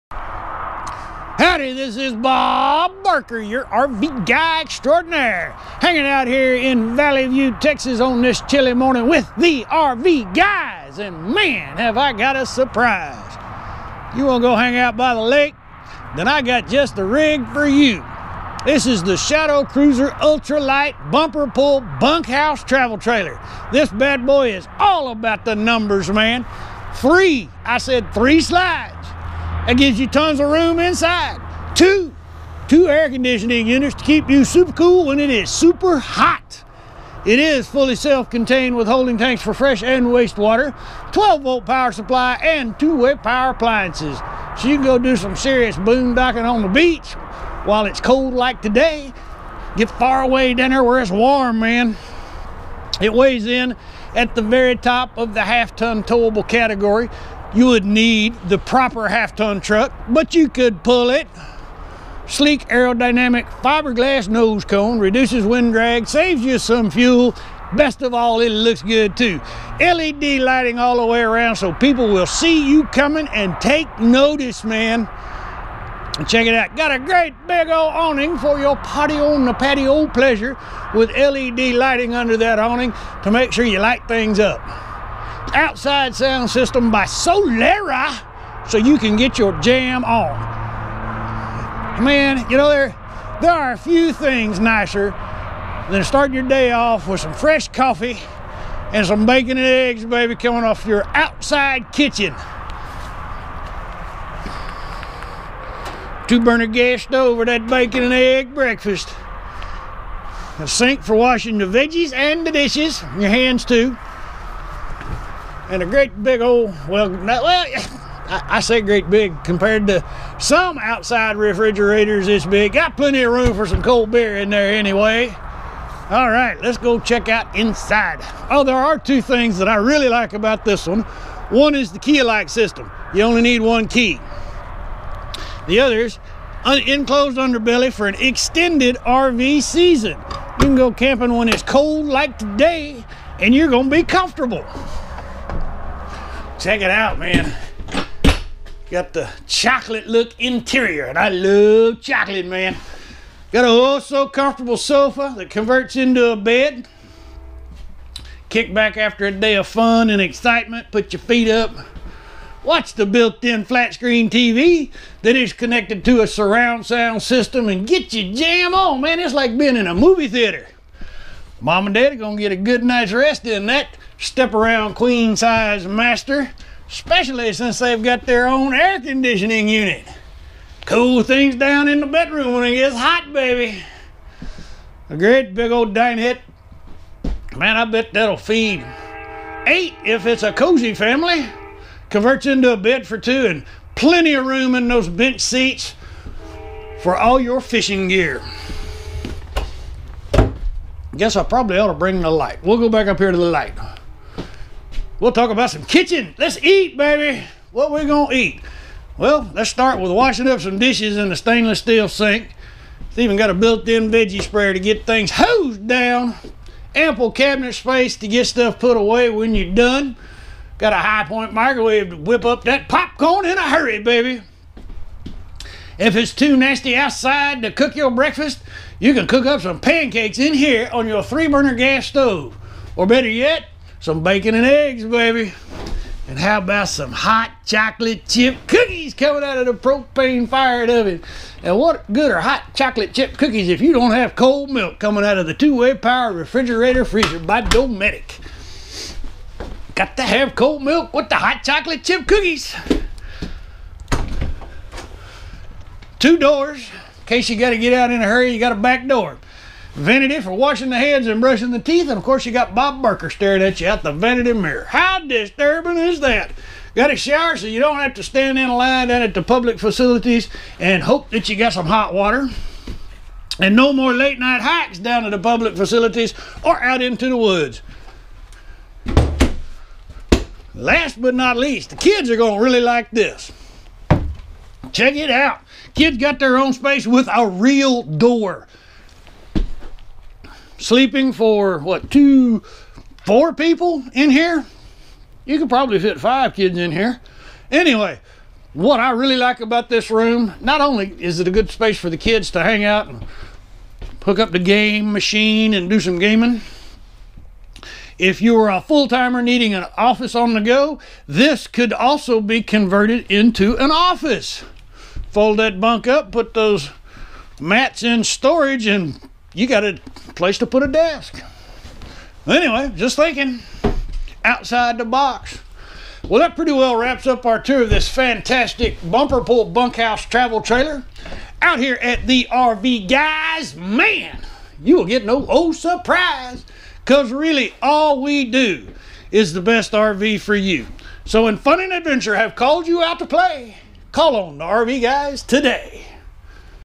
WOW! Your Kids Will Love You For This! 2017 Shadow Cruiser313BHS
When the call of the wild has your children tearing up the house and driving you crazy! you now have a remedy! With sleeping for up to ten this bumper pull bunk house is just the ticket to calm the wild beasts! Just imagine, sitting fire side telling scary stories next to a remote mountain stream with the stars shinning and the water babbling on the rocks all while munching on Smores and sipping hot coco! Those are the memories that will last a life time!
  With holding tanks for fresh/waste water, 12 volt power supply and two way power appliances you are fully self contained and can set up camp way out in the back of beyond and still have the comforts you require! Thanks to having a full bath with an on-board shower your crew will camp in clean comfort no matter how much dirty fun you have!  Just imagine how fun and relaxing it will be to boondock in remote and far away places where there are no modern distractions and large crowds! Meal prep is a snap thanks to having two kitchens! One inside and one outside! The spacious bunk room has sleeping for up to four and is ideal for the kids to have their very own space.
  Now is your time for fun, Excitement, travel and adventure! Grand memories are waiting for you just down the road! You really Dont want to wait! Tomorrow never comes and you can never turn back The clock! Your family’s new life of adventure and fun is just waiting for you right here in this bumper pull bunk house travel trailer!
Now that adventure and fun have called you out to play!